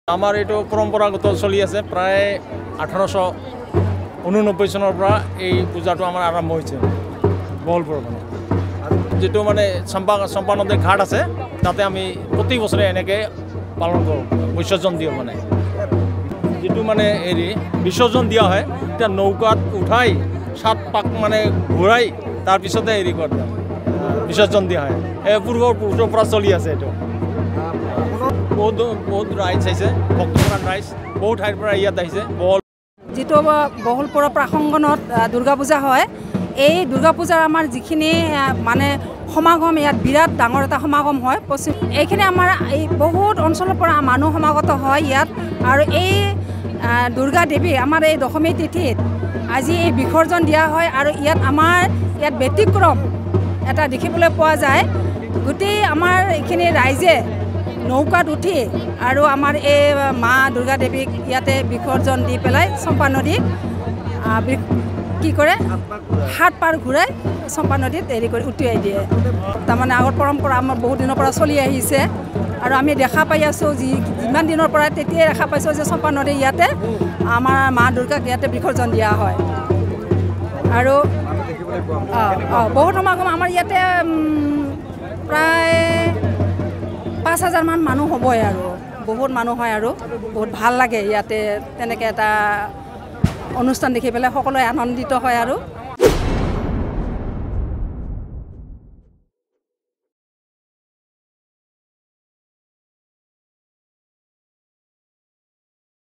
বহলপুৰৰ চম্পাৱতী নৈত মা দুৰ্গাৰ প্ৰতিমূৰ্তি বিসৰ্জন | Narad Live
ধুবুৰী জিলাৰ বহলপুৰৰ চম্পাৱতী নৈত মা দুৰ্গাৰ প্ৰতিমূৰ্তি নৌকাত উঠাই সাত পাক পদক্ষিণ কৰি বিসৰ্জন দিয়ে।